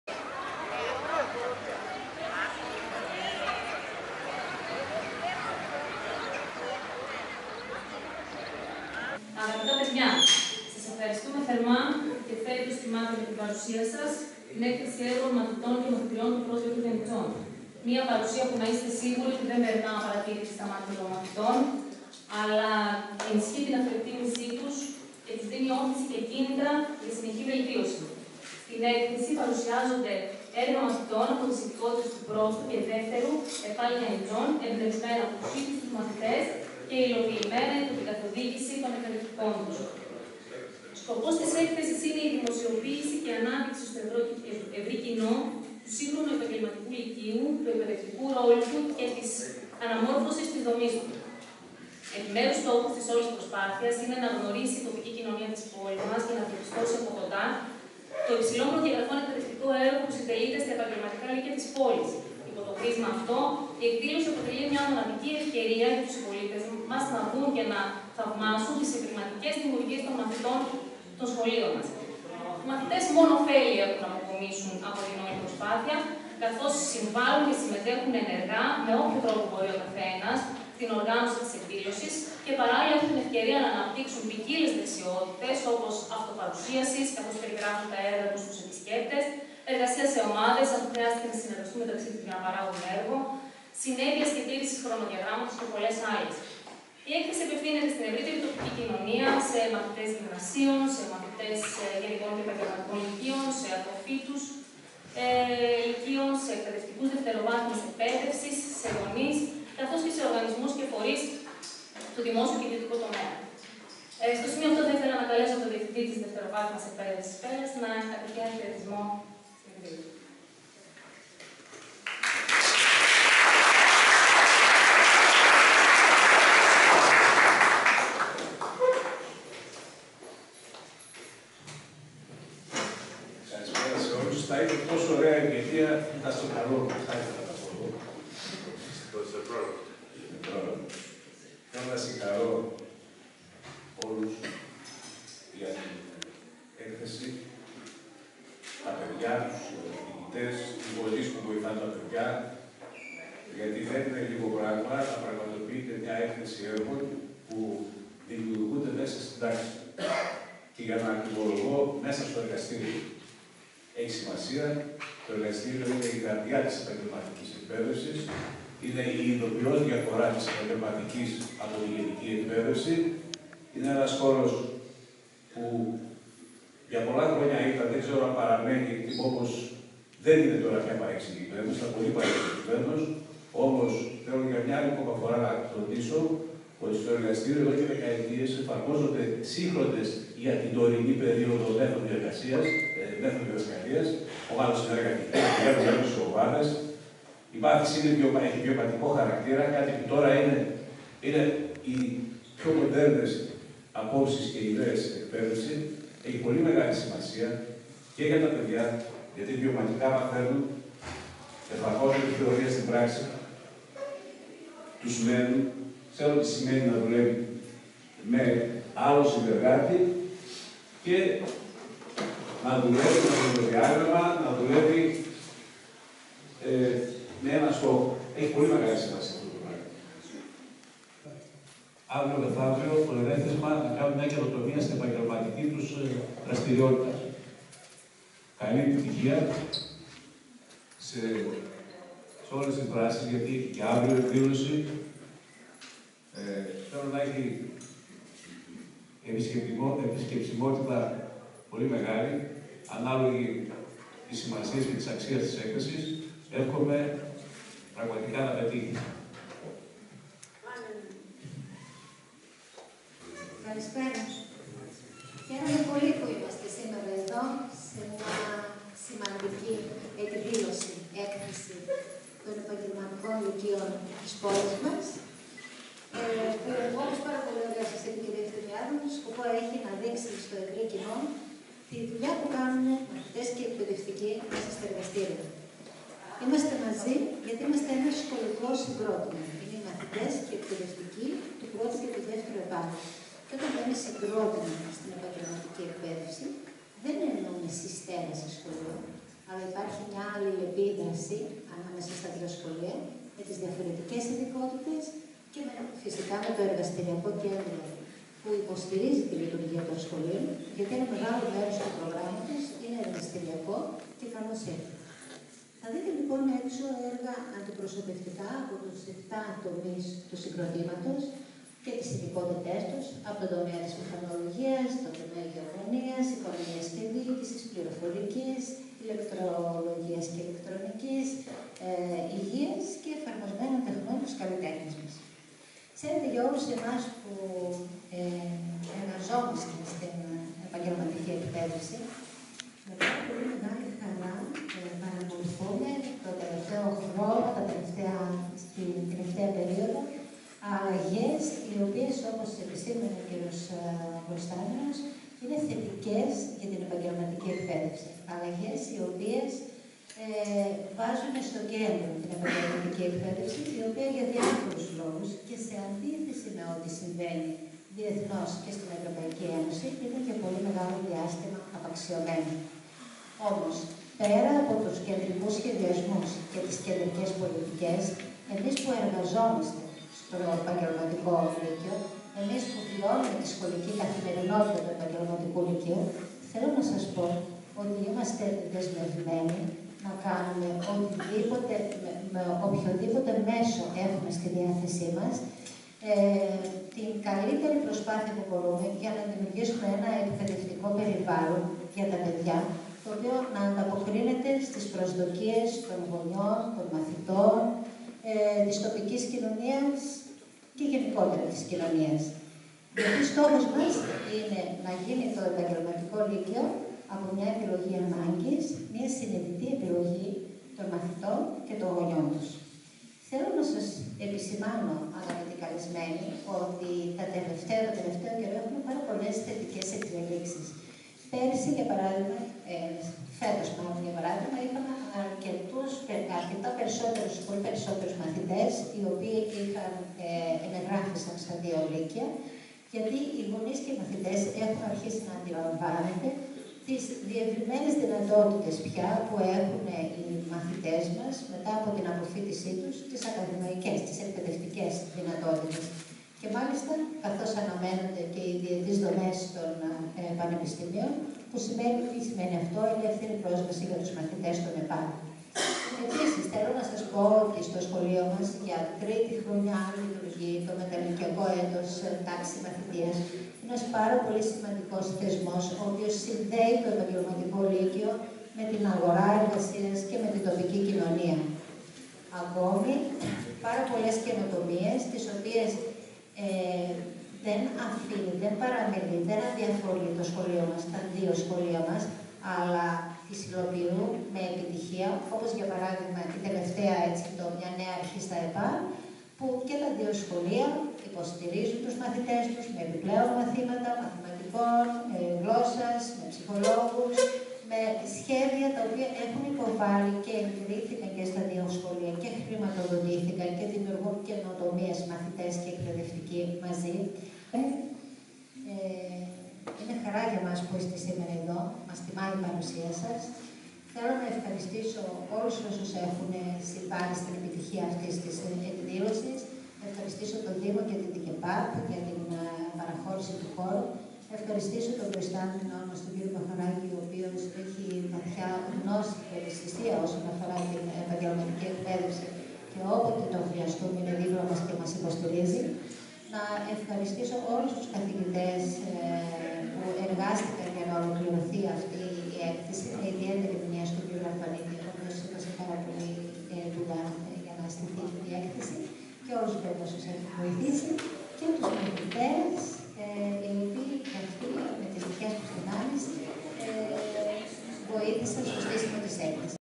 ΕΚΘΕΣΗ "ΤΟ ΕΠΑΛ ΔΗΜΙΟΥΡΓΕΙ" (Νο1) ΟΜΙΛΙΕΣ - ΓΙΑΝΝΙΤΣΑ 13/5/2019
ΒΙΝΤΕΟ Αντώνης Παπαδόπουλος (ΣΥΜΒΑΙΝΕΙ ΤΩΡΑ) - Πνευματικό Κέντρο Γιαννιτσών.